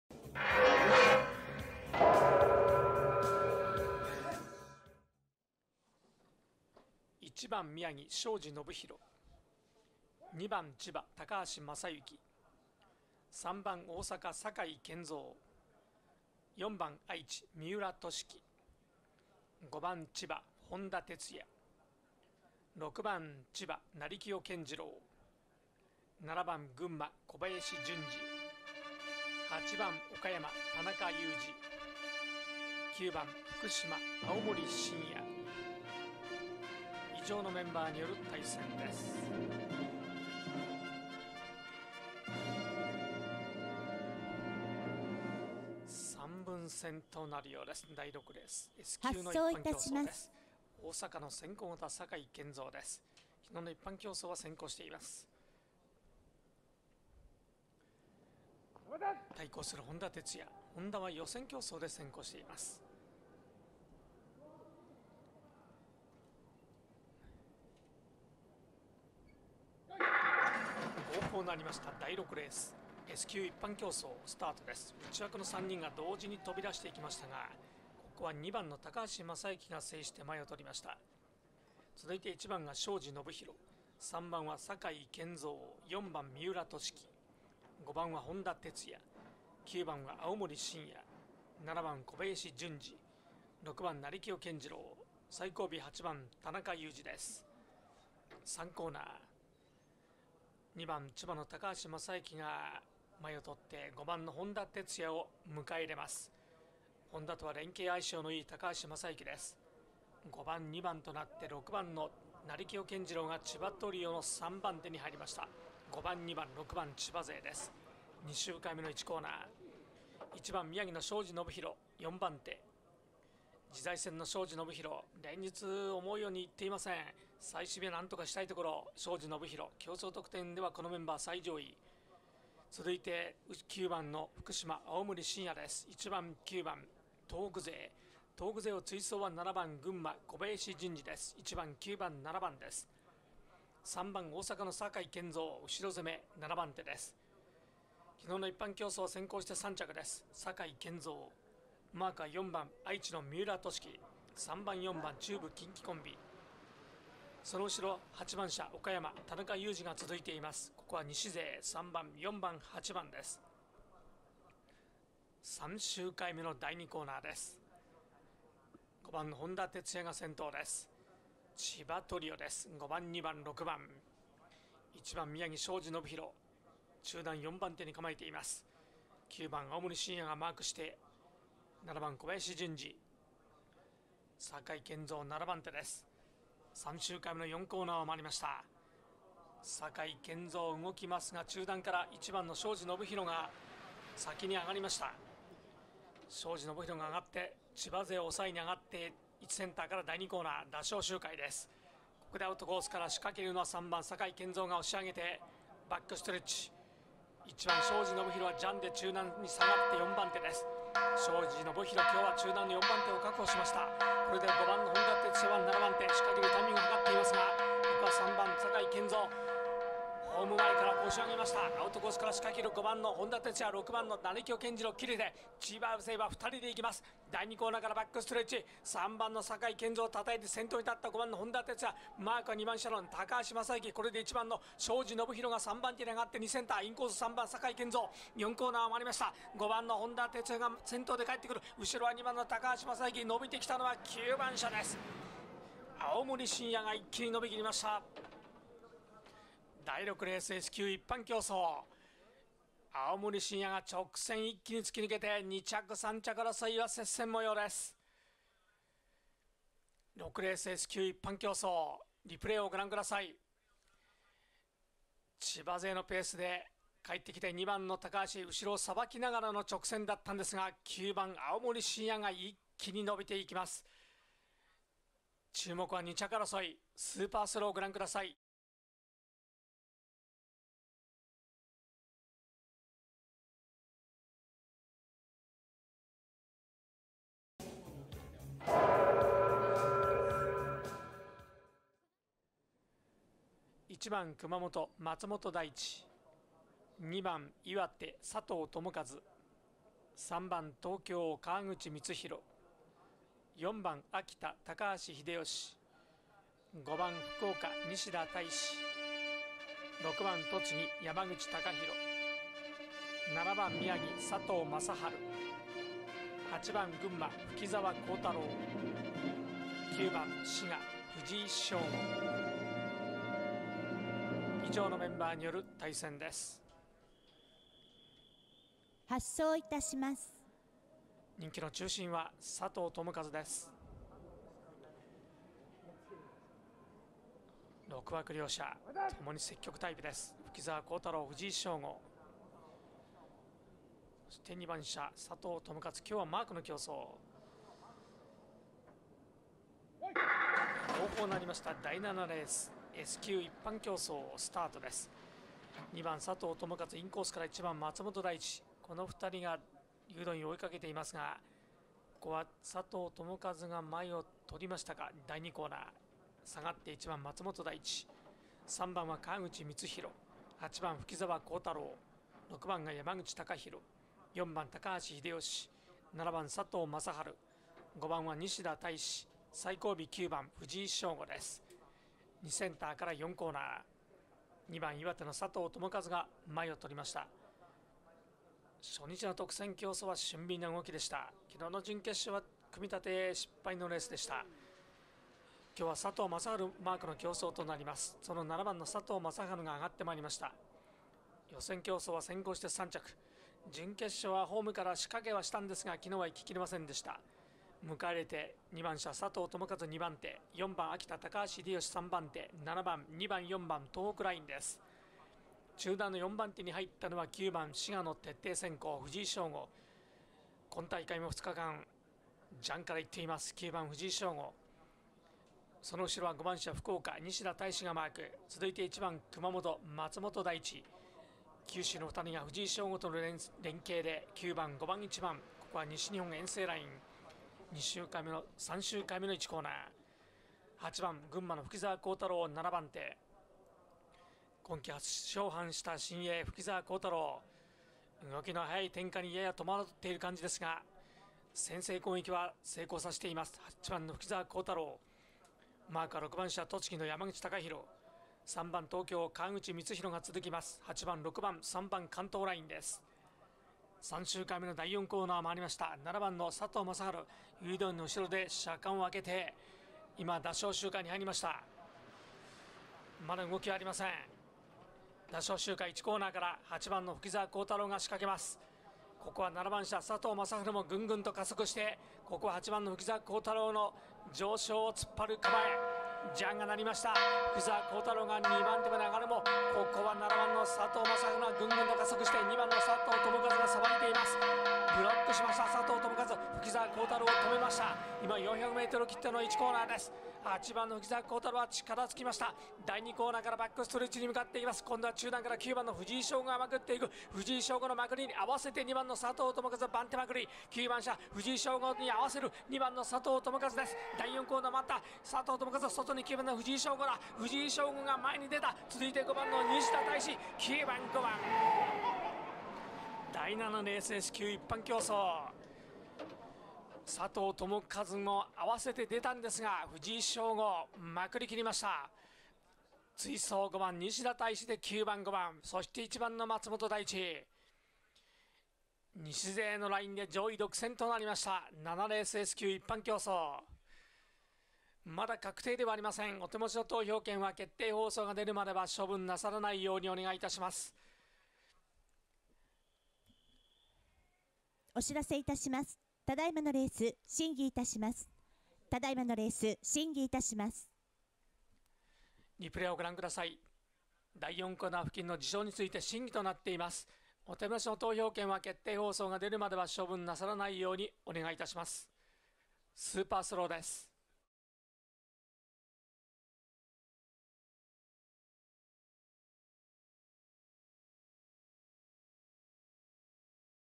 20190718【平塚競輪】FⅠ 湘南クリーンサイクル杯 7/18（木）【6R、7R S級一般】
【平塚競輪】FⅠ 湘南クリーンサイクル杯 7/18(木)
【6R S級一般】
1番車：庄子　信弘
2番車：高橋　雅之
3番車：酒井　拳蔵
4番車：三浦　稔希
5番車：本多　哲也
6番車：成清　謙二郎
7番車：小林　潤二
8番車：田中　勇二
9番車：青森　伸也

【7R S級一般】
1番車：松本　大地
2番車：佐藤　友和
3番車：川口　満宏
4番車：高橋　秀吉
5番車：西田　大志
6番車：山口　貴弘
7番車：佐藤　雅春
8番車：蕗澤　鴻太郎
9番車：藤井　昭吾